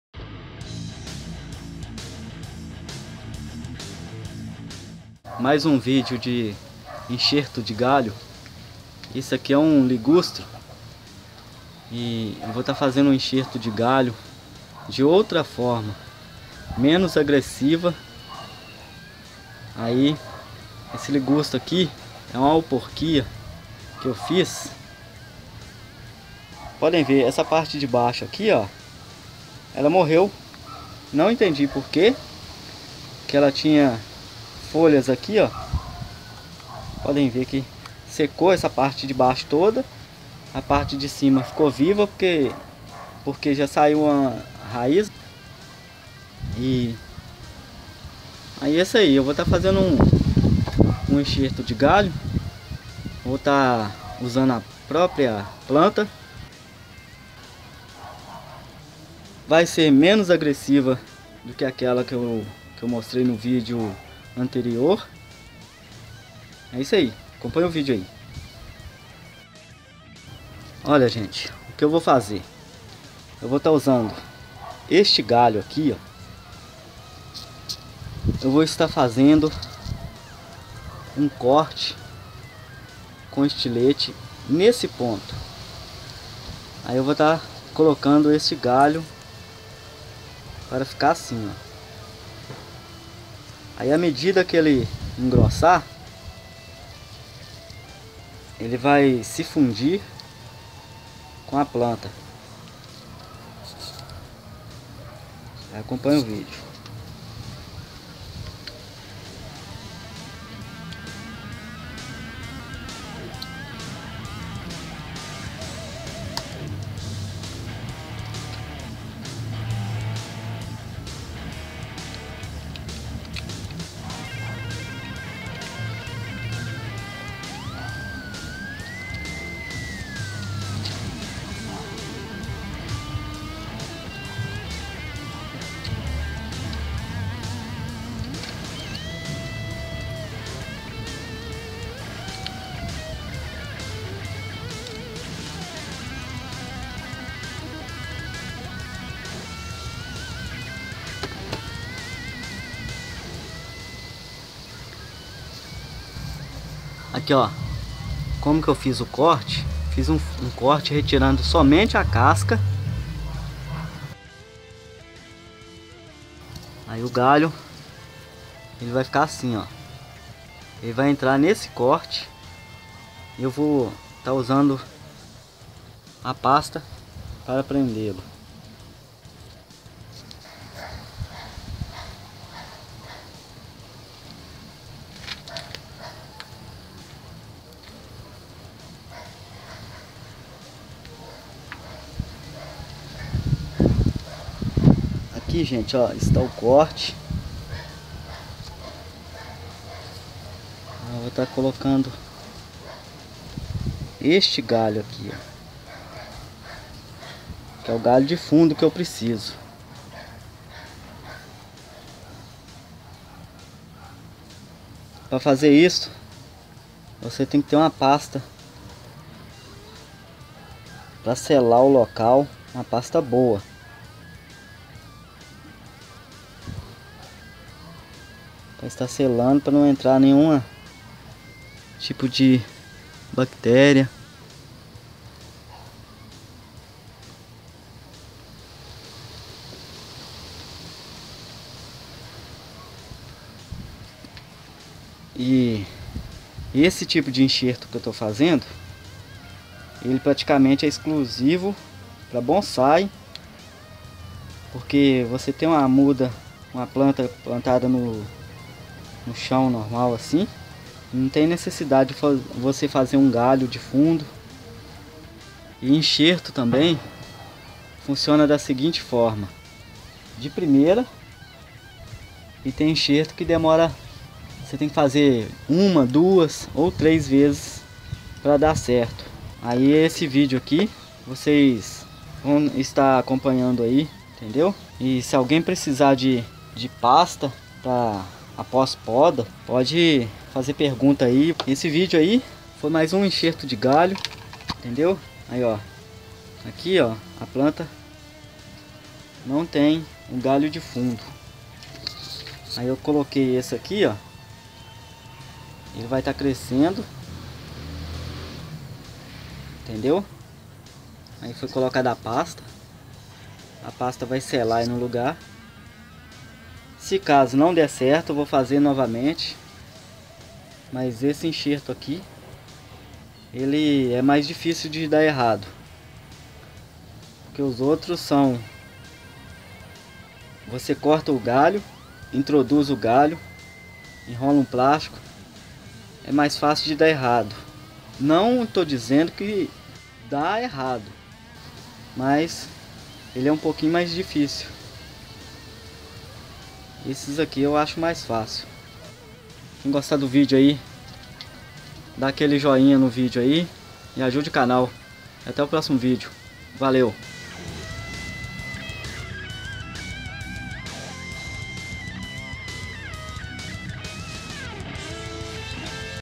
2° Enxerto de galho ( ligustrum )
Este é o segundo vídeo sobre enxerto de galho para bonsai este é uma muda de ligustrum que não tinha galho de fundo,compartilhem e inscreva -se , PASTA VENDAS , PARA COMPRAR PASTA